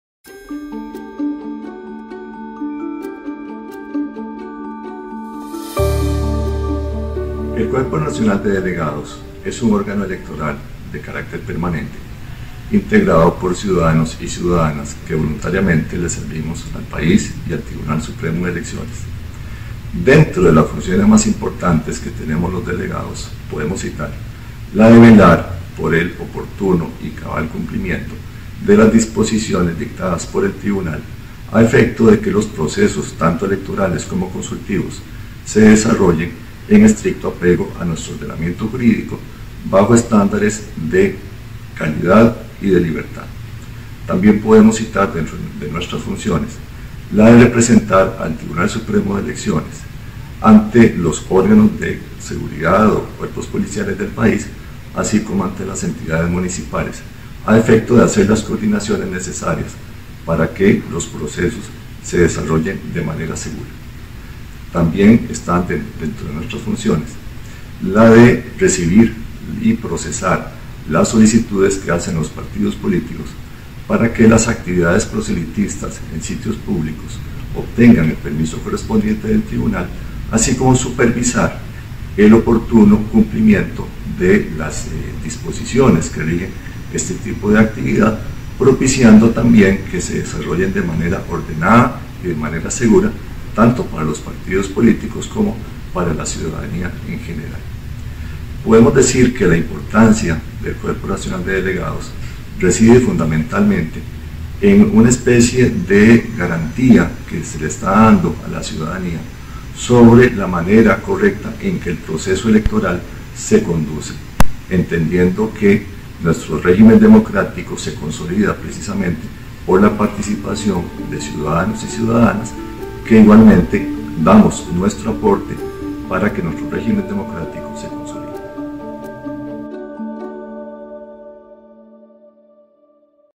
Cuerpo Nacional de Delegados
Entre los agentes electorales que colaboran con el TSE en la vigilancia del proceso electoral, se encuentra el Cuerpo Nacional de Delegados.